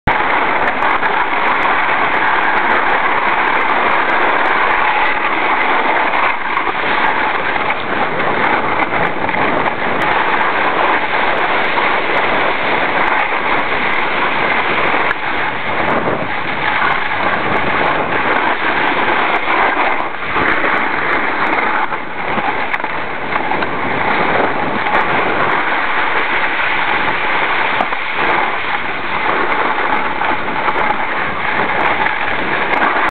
Rick snowboarding
after 4 years of no riding, i felt pretty good about this ugly run through the pipe at bristol.